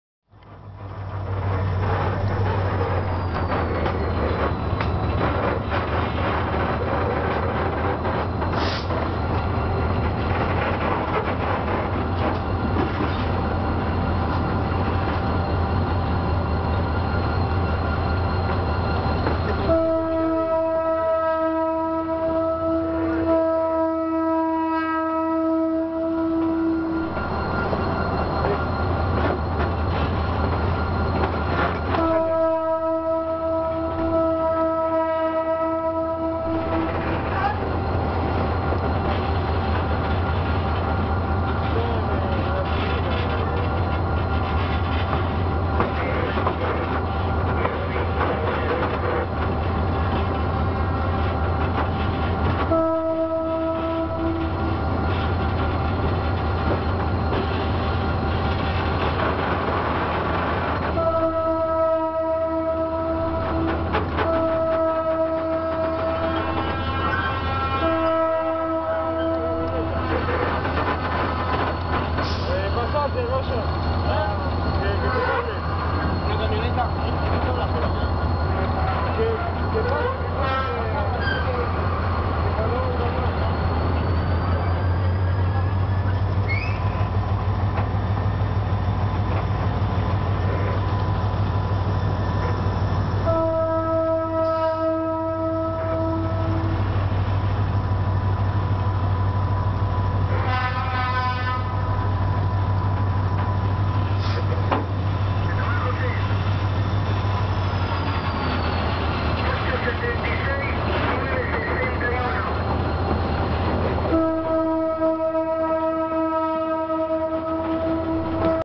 Viaje en GT22 9020 con PC Vacio + Chapas Salida Emp.Norte - Ferrosur roca
en Horas de la tarde ya dejado los PC Cargados y acoplado PC Vacios y las chatas que trajo NCA sale rumbo a Kilo 5, en el costado en el paneo que hago veo la GAIA 5272 de TBA que vino del Tunel FCO hacia TBA Mitre